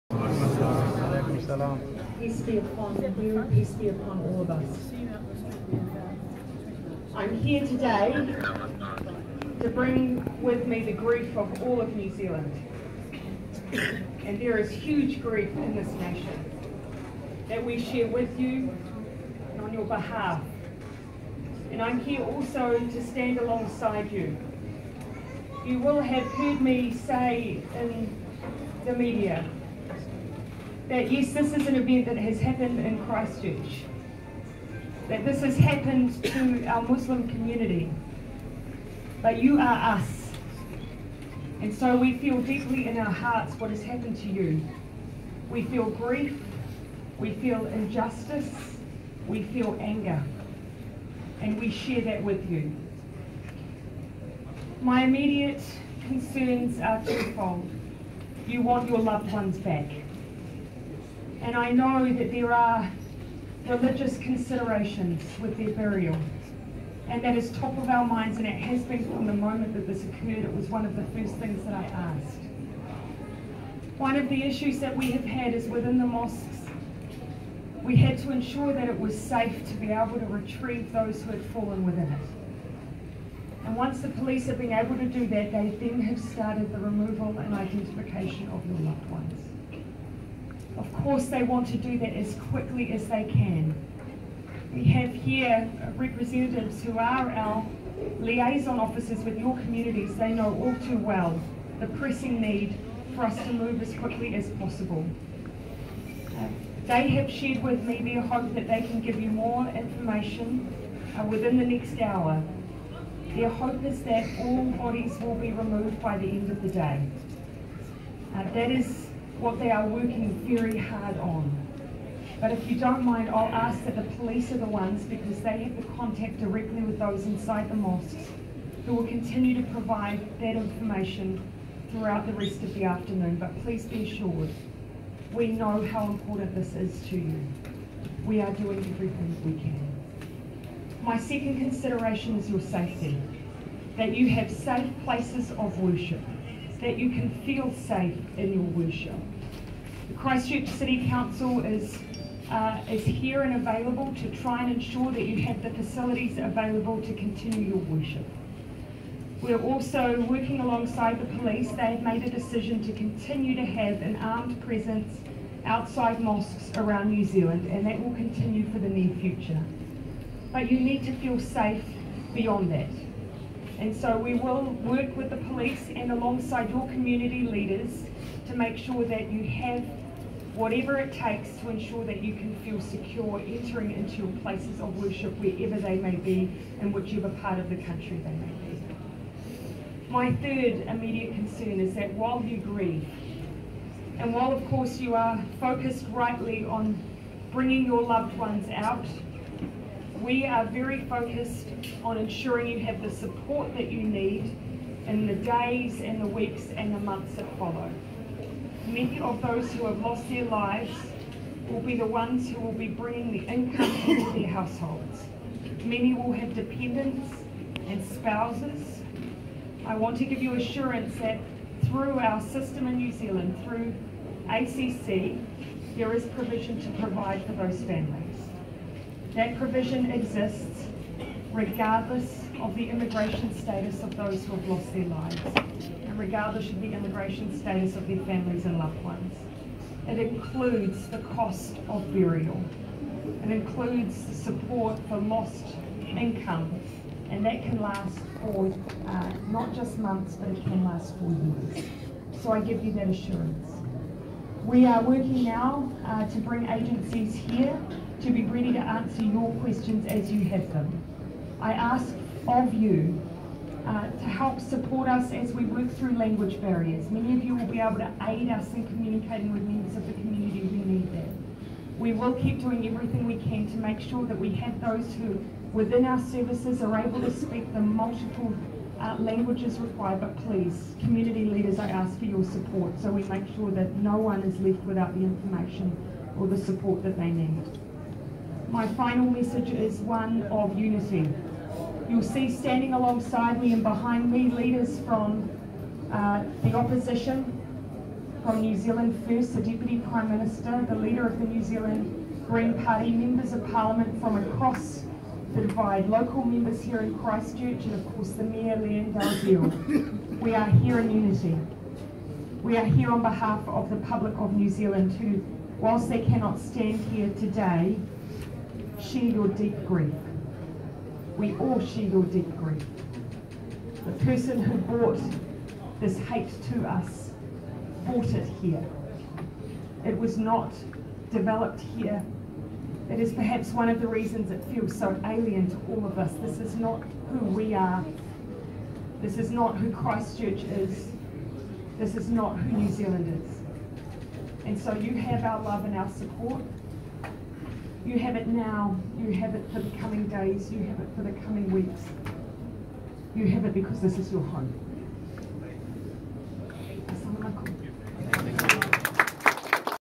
Exclusive by Jacinda Ardern at Mosque
Christchurch mosque shootings: Accused gunman sent manifesto to Prime Minister Jacinda Ardern's office before attack . Excellent Gesture by PM.